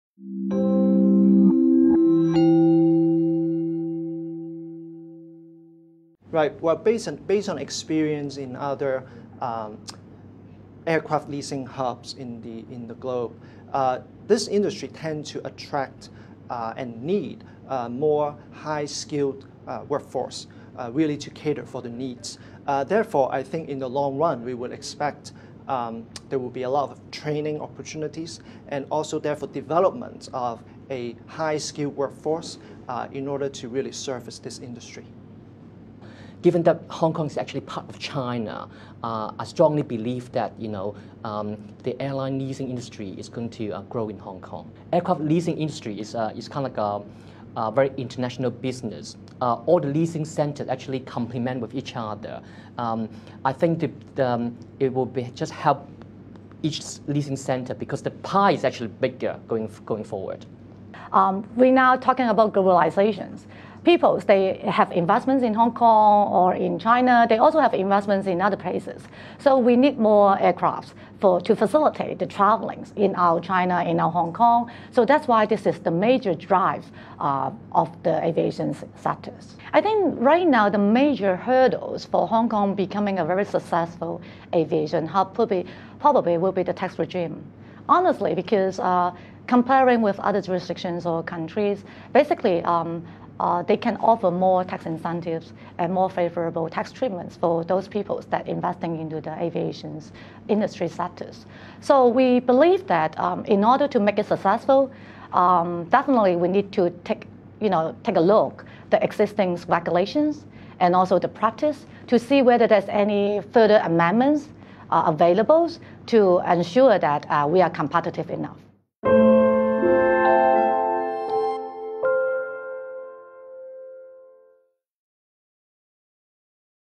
Aviation Leasing: Tax reforms will boost HK industry and create jobs
Our subject matter experts give their views on the potential for the aircraft leasing industry in Hong Kong, following the Chief Executive's Policy Address of 14 January 2015.  For our viewpoints on the HK Budget,